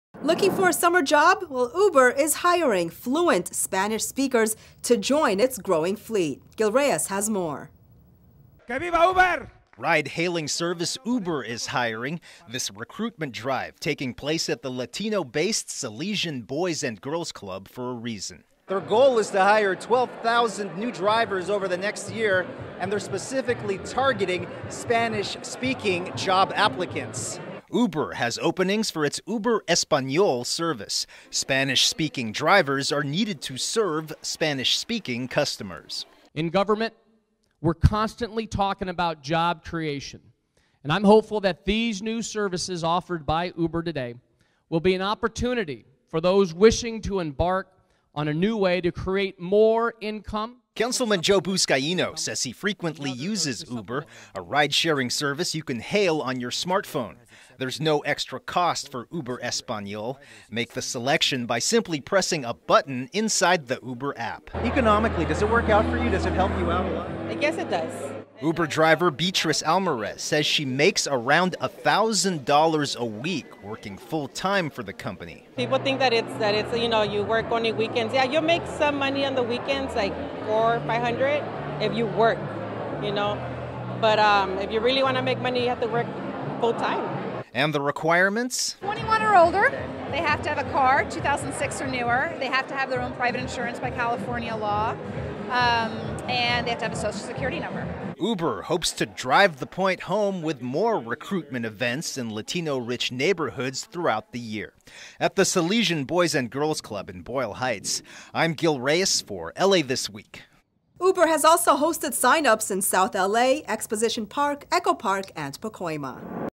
Uber Espanol
Uber is hiring and their goal is to hire 12, 000 new employees over the year, with their main aim for employment being Spanish speaking applicants.

Gil Reyes explains
Cameraman: Tom Kuwahara